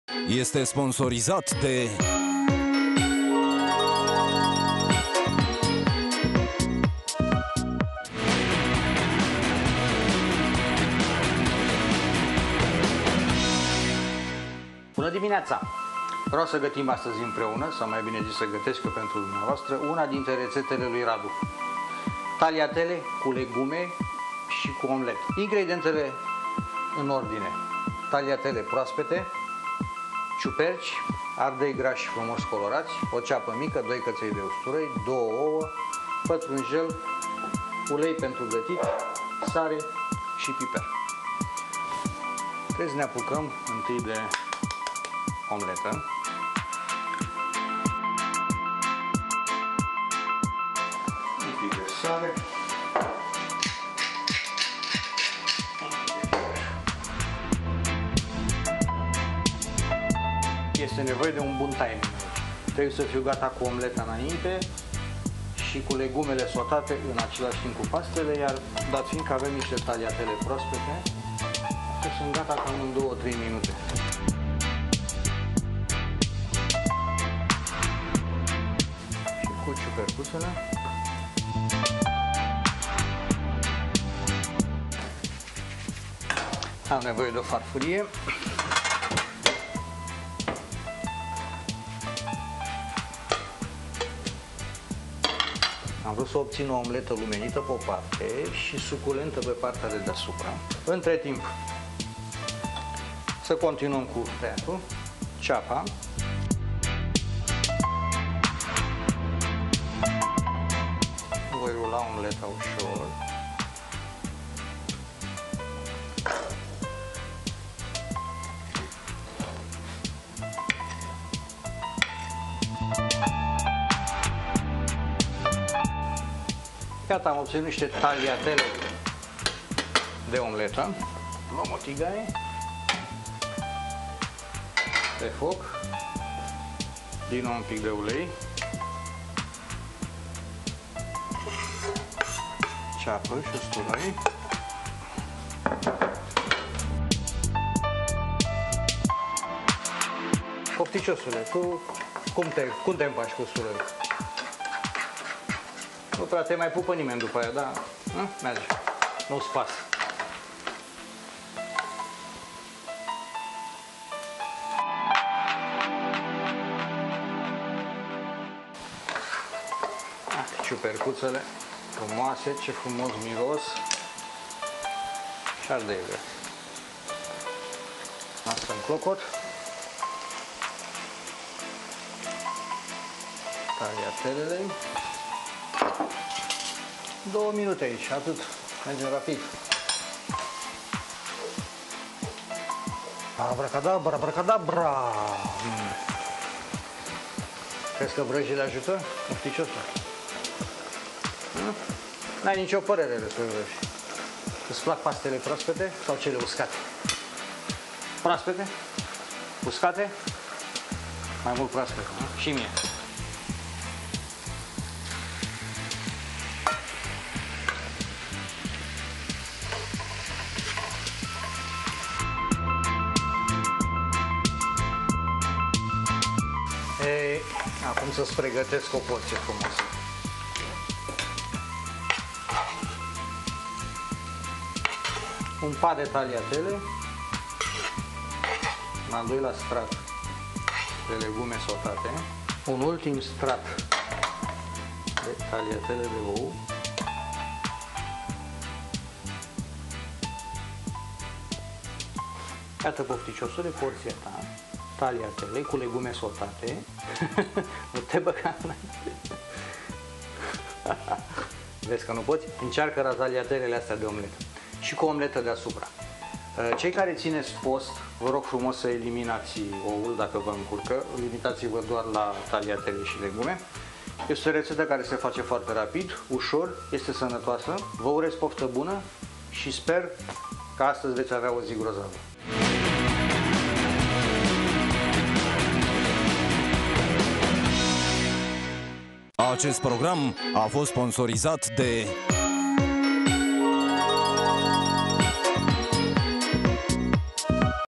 Tagliatelle cu legume si omleta.flv
Preparatul este plin de culoare si bine echilibrat nutritional. Se prepara, de asemenea, foarte rapid si este delicios. Il puteti servi la cina, sau ca pranz usor, mai ales in lunile de vara, cand legumele sunt in plin sezon.